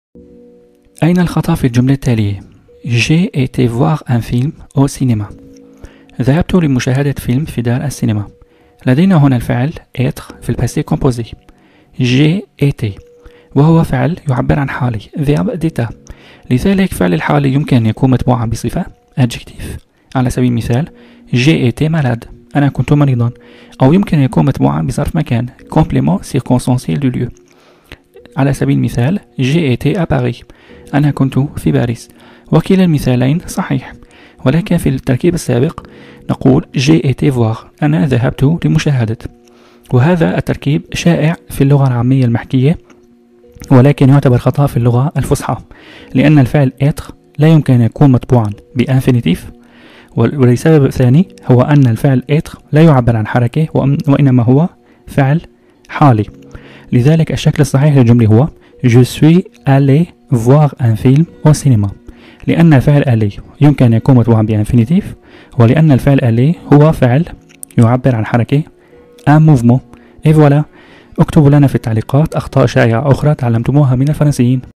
أين الخطأ في هذه الجملة؟ - خطأ يقع به الكثير الفرنسيين 😱
تجنب هذا الخطأ الذي يقع به الفرنسيين 😱